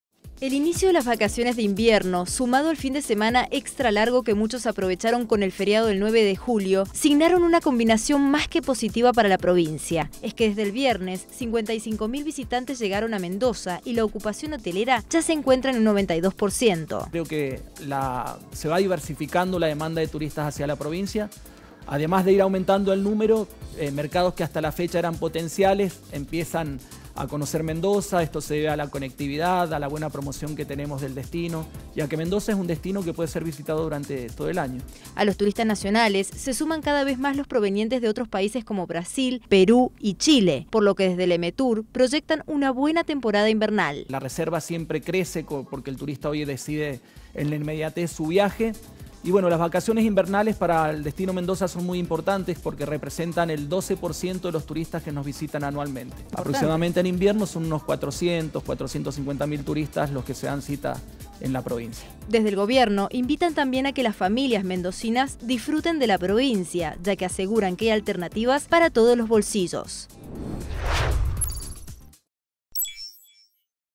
Mendoza tuvo más del 90% de ocupación hotelera
Desde el viernes llegaron a la provincia 55 mil visitantes ¿Qué significa esto?